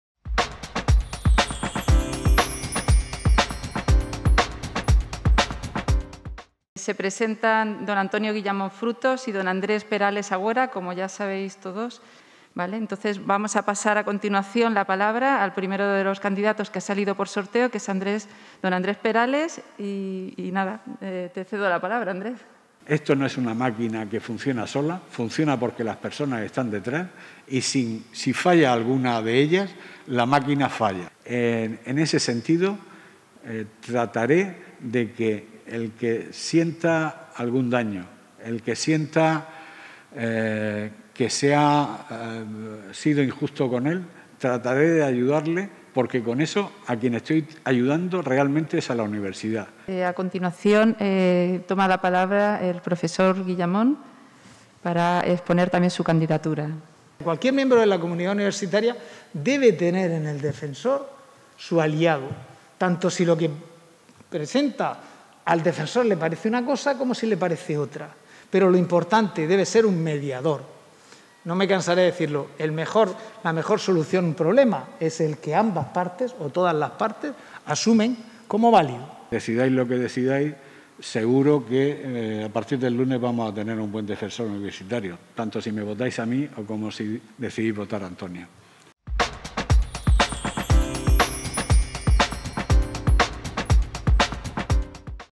Antonio Guillamón y Andrés Perales se presentan para defensor universitario
Ambos profesores de la UPCT explicaron al Claustro su experiencia, motivación e ideas para ejercer el cargo en defensa de la comunidad universitaria